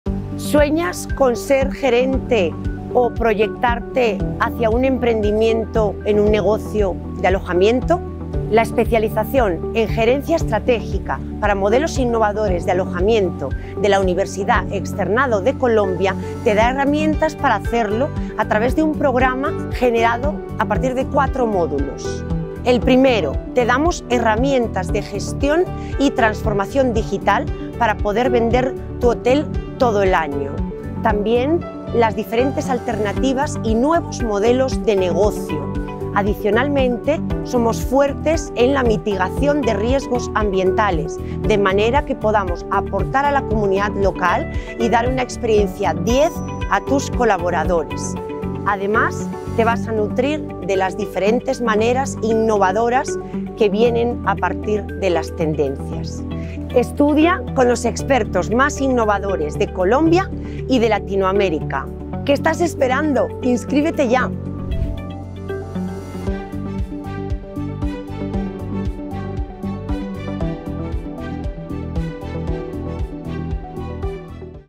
Especialización Gerencia Estratégica para Modelos Innovadores de Alojamiento
Adquiere herramientas de gestión estratégica y transformación digital que te ayuden a vender el hotel todo el año, entender los nuevos modelos de negocio y distribución y mitigar los riesgos medioambientales de tu alojamiento al tiempo que aportas al desarrollo de las comunidades y tus colaboradores.

Código SNIES: 109238
Resolución 16763 del 27 de diciembre de 2019 por 7 años
Modalidad: Presencial, Bogotá